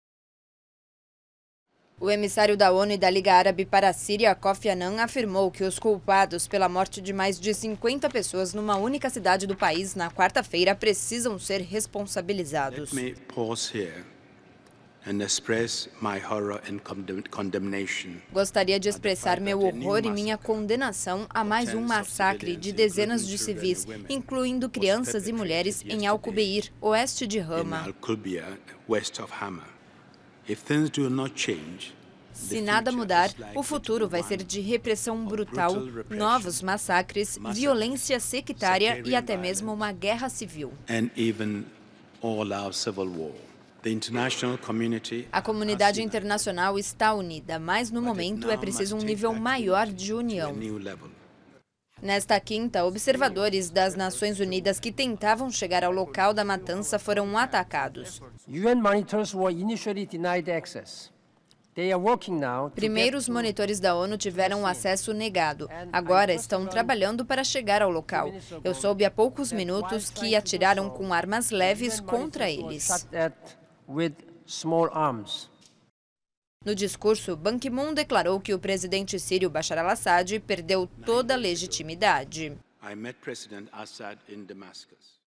Ataque contra a ONU na Síria
Observadores da ONU na Síria foram atacados com armas de fogo, nesta quinta-feira. O Secretário-geral da ONU, Ban Ki-moon, declarou que o presidente sírio, Bashar al-Assad, perdeu toda a legitimidade.